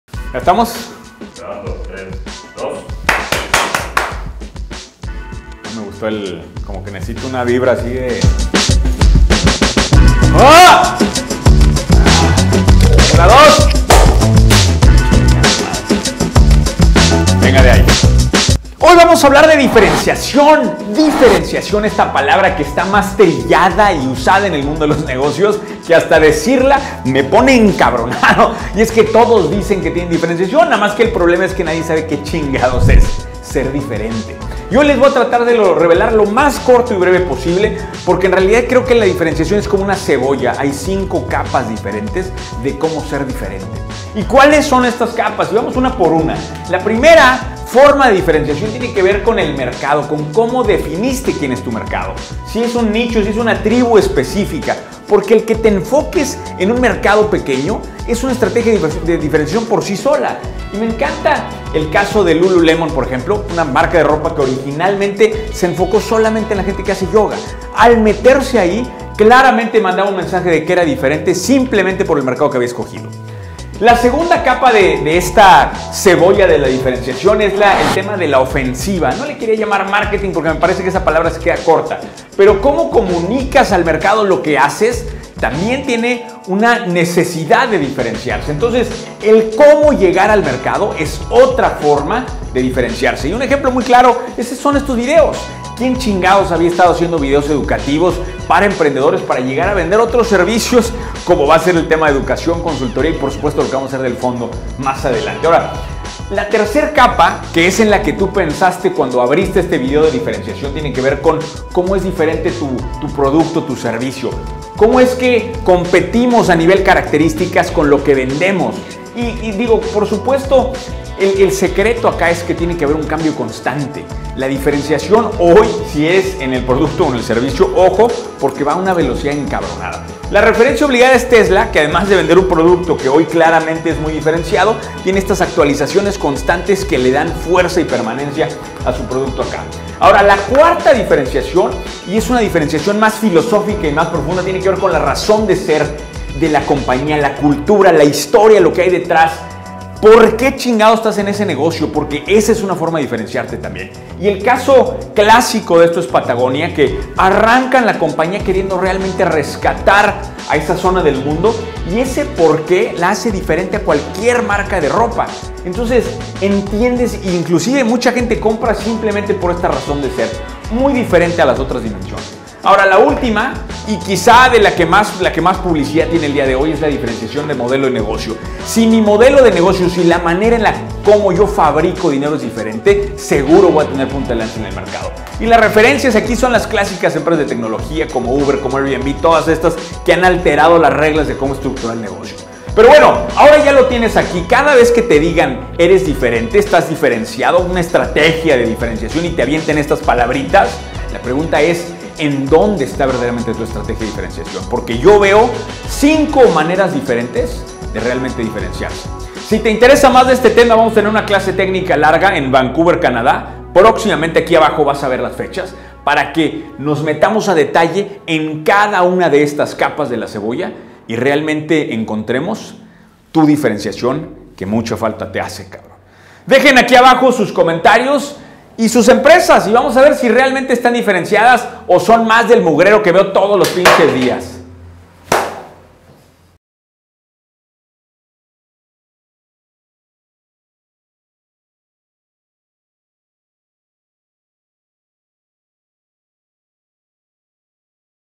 A TU NEGOCIO LE HACE FALTA CEBOLLA
Cada vez que te digan que eres diferente pregúntate ¿En donde está verdaderamente tu estrategia de diferenciación?
Aquí te dejo cinco maneras de como diferenciarse.

ESTO ES LECCIONES EN CRECIMIENTO EXPONENCIAL.

PRÓXIMAMENTE EN VANCOUVER, CANADA.